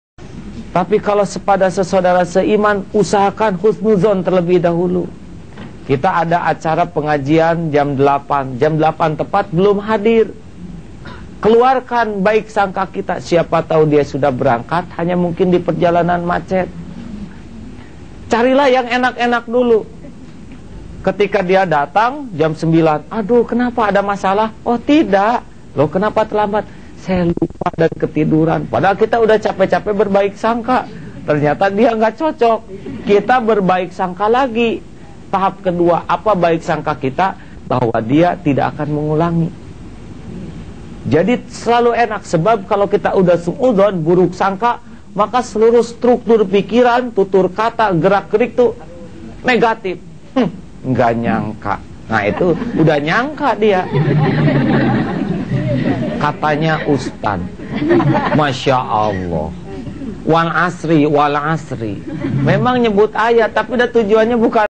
With Love: Aa Gym Jangan Buruk Sangka
Yang suka buruk sangka nih dengerin ceramahnya Aa Gym